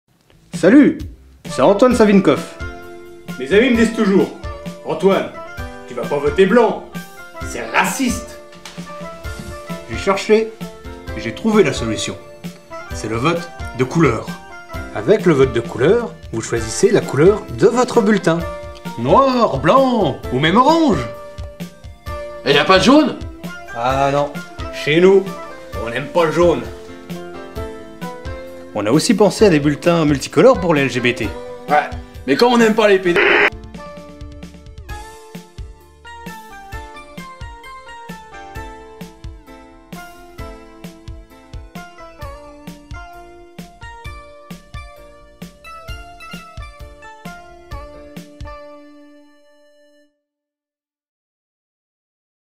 Macron - vote de couleur - ben et sav - présidentielles 2017
Ben et Sav vous présentent ce sketch à prendre au second degré évidemment.